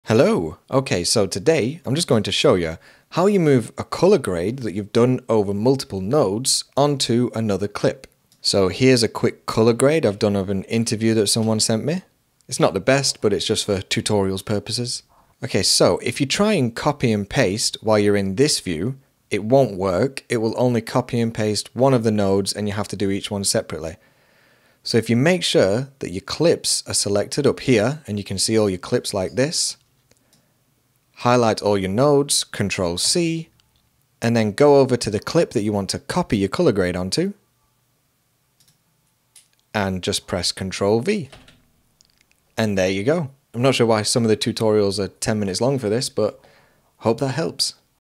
How To COPY MULTIPLE NODES in Davinci Resolve 18
A quick overview on copying multiple nodes in Davinci Resolve 18 from one clip to another, a simple task that is hidden since it doesn't work in all of the views in the colour tab (for some reason)